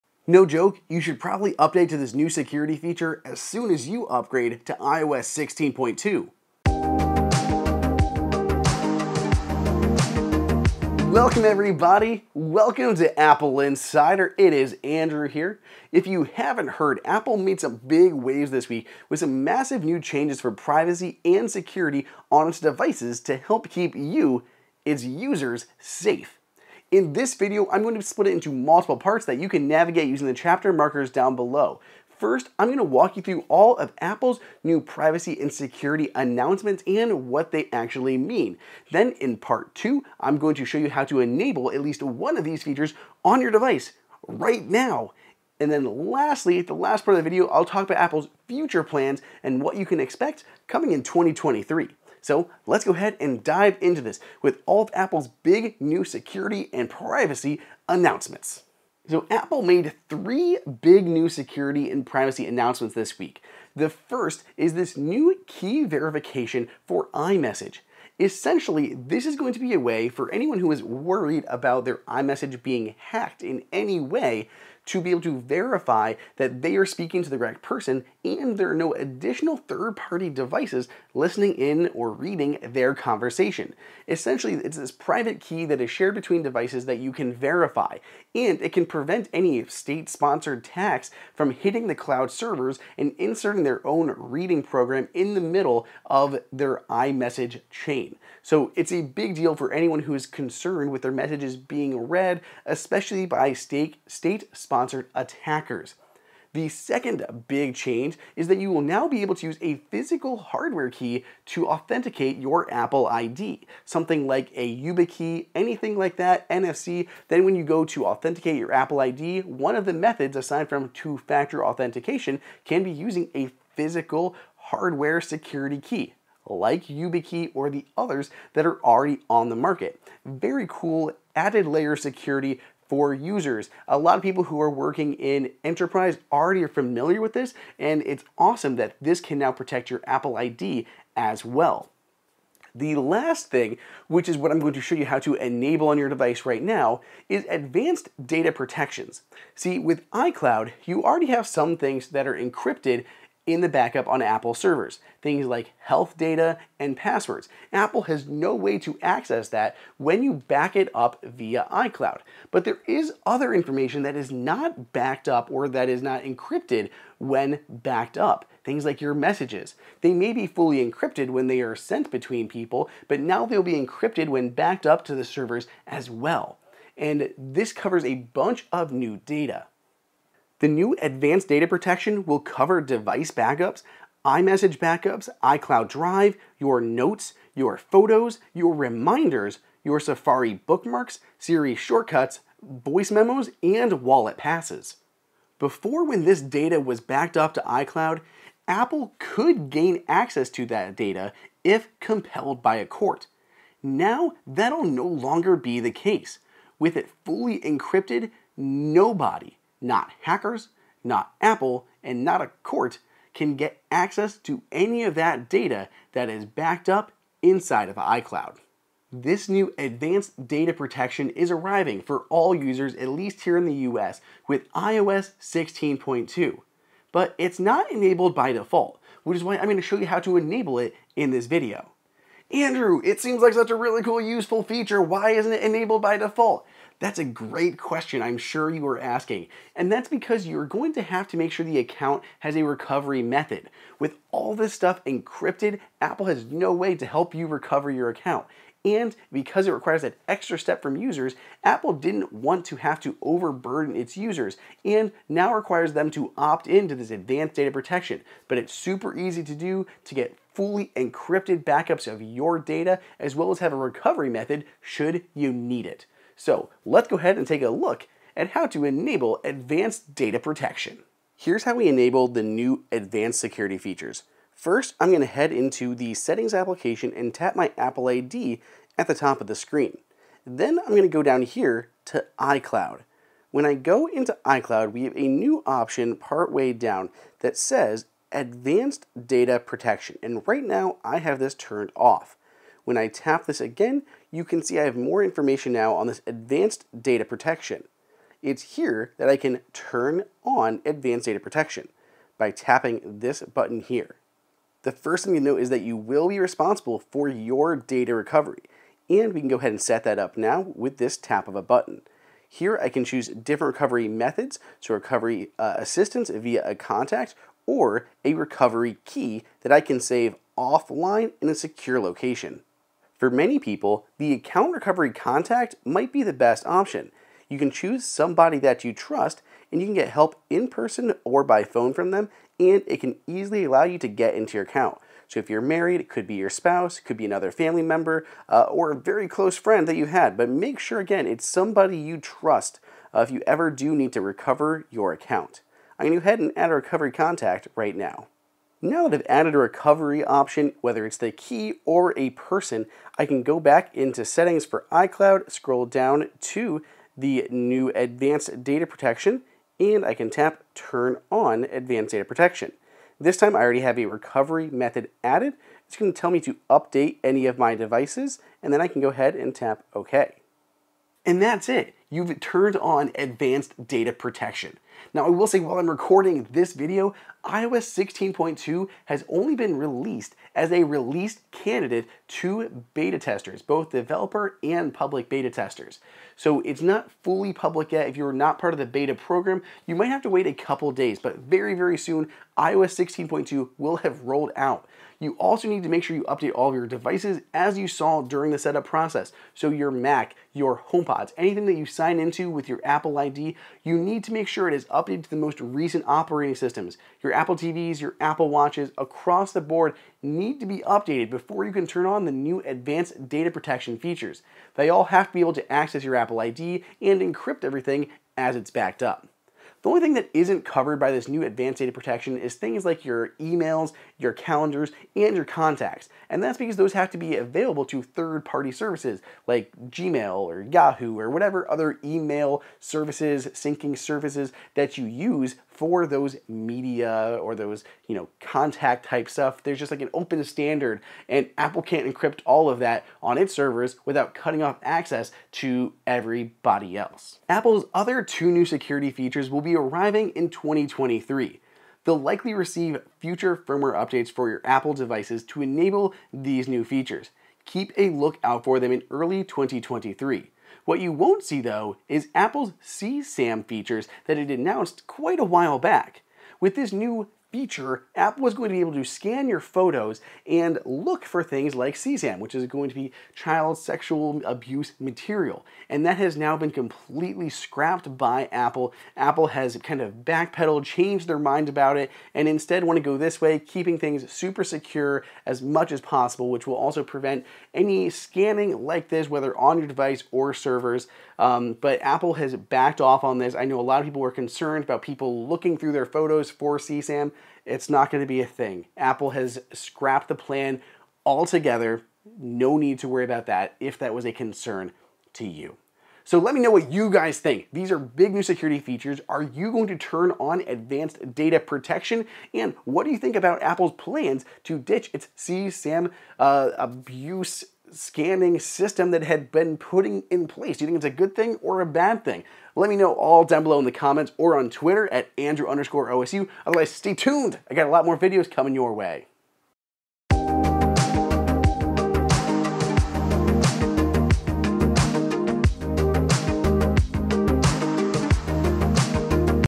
Enable This Security Feature RIGHT NOW in iOS 16.2!
Apple has big new security changes coming to help protect you and your data. Here are the newest features, when they'll be available, and how to enable one of them RIGHT NOW.

Chapters ➡
0:00 Intro
0:58 3 New Security Announcements
4:31 Enabled Advanced Data Protections
7:62 Security Updates in 2023